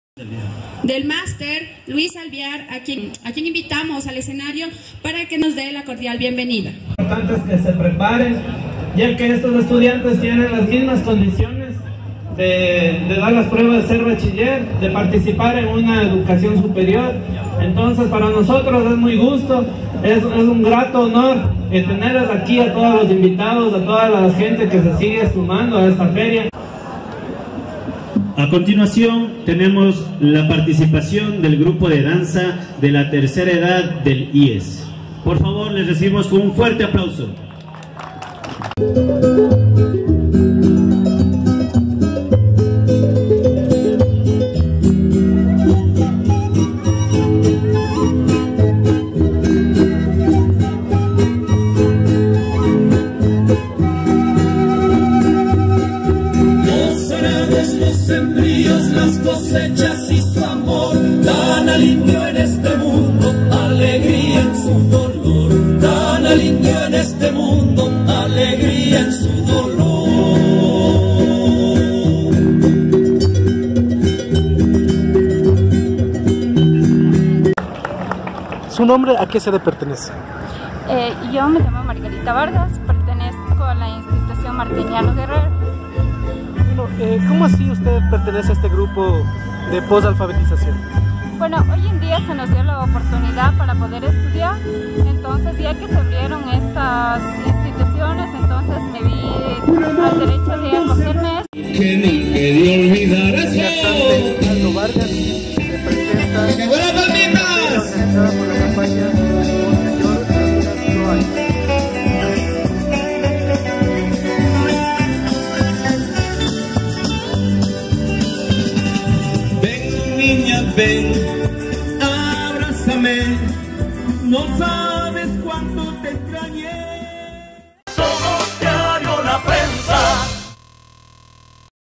Las personas adultas con escolaridad inconclusa se presentaron este viernes en una feria
Las personas adultas con escolaridad inconclusa se presentaron este viernes en una feria para promover la campaña Todos ABC: Monseñor Leonidas Proaño, de Alfabetización, Educación Básica y Bachillerato.